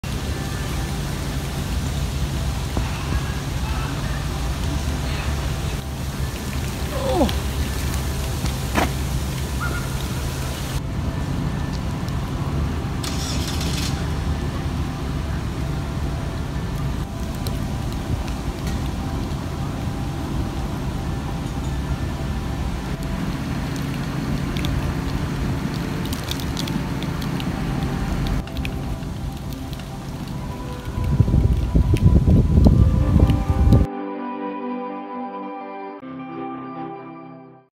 Search expands for missing 5-year-old girl
This is the corner of Springfield Boulevard and Ivy Street. An Amber Alert was issued for 5-year-old Taylor Rose Williams.